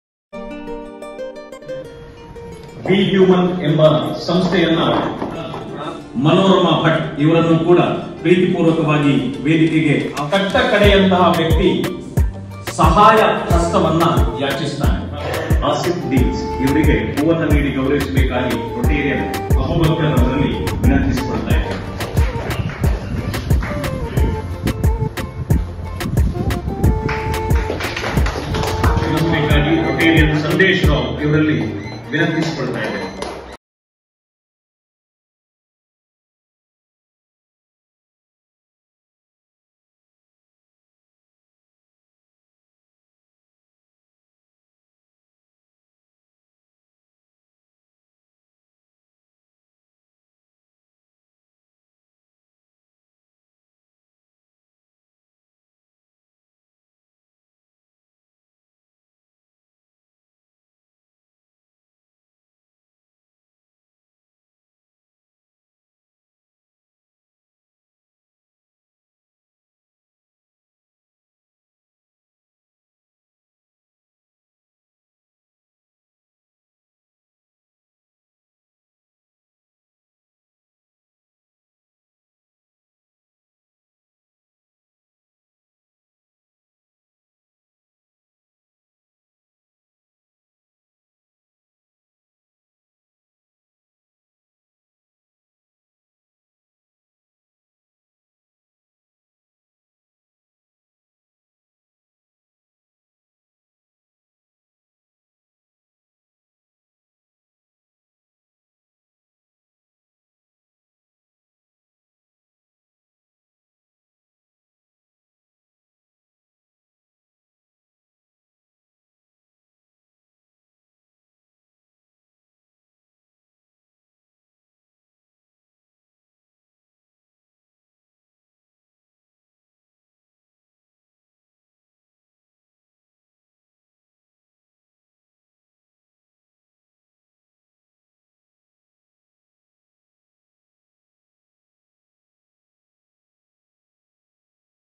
B-Human & Yenepoya Hospital Mangalore Successfully Organized Free Medical Health Camp.
B-Human & Yenepoya Hospital Mangalore In Association With Rotary Club Belthangady, Prerana Sanjeevini Mahila Okkoota Ujire, Grama Panchayath Ujire. Successfully Organized Free Medical Health Camp @ SDM Aided School Ujire On 21st Jan 2023

𝙒𝙚𝙗: b-human.in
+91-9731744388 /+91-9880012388